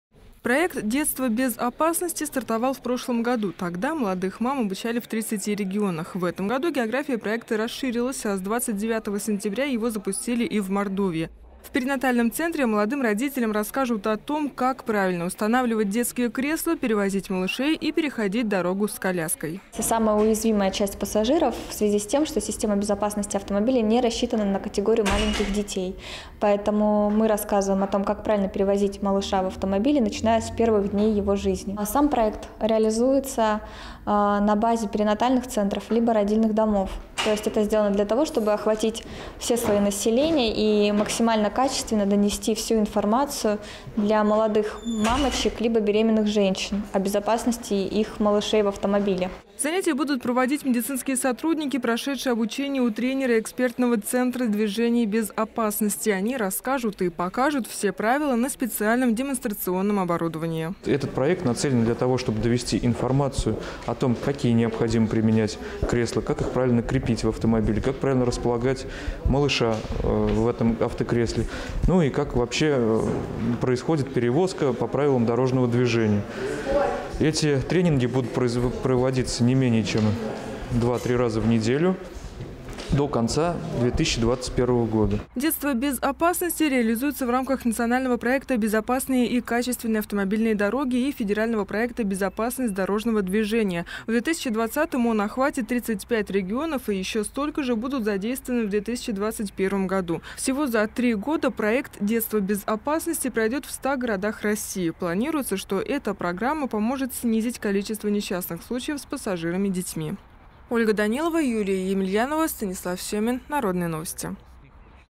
Проект «Детство без опасности»
В Мордовии стартовал   федеральный проект «Детство без опасности». В его рамках молодых родителей обучат правилам перевозки детей в автомобиле. Тренинги проведут на базе республиканской центральной клинической больницы.